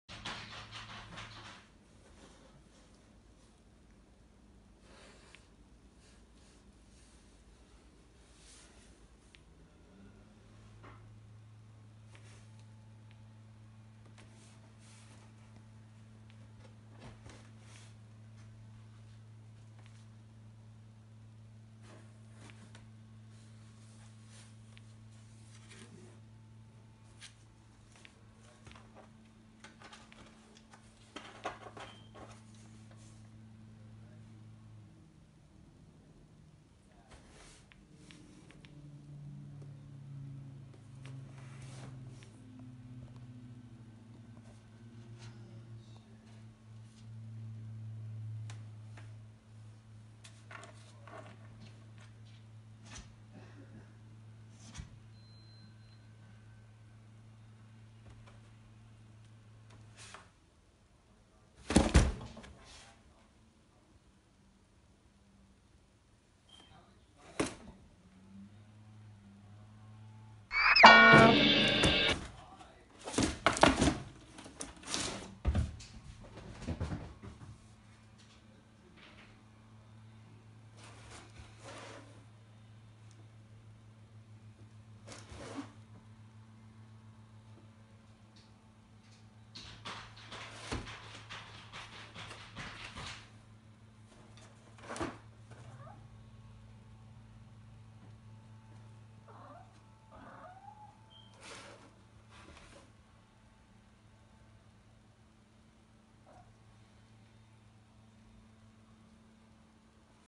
Willy & Cuco 😼😾
Cuco está muy a gusto en la caja y Willy lo corre, para quedarse él con ella 📦😸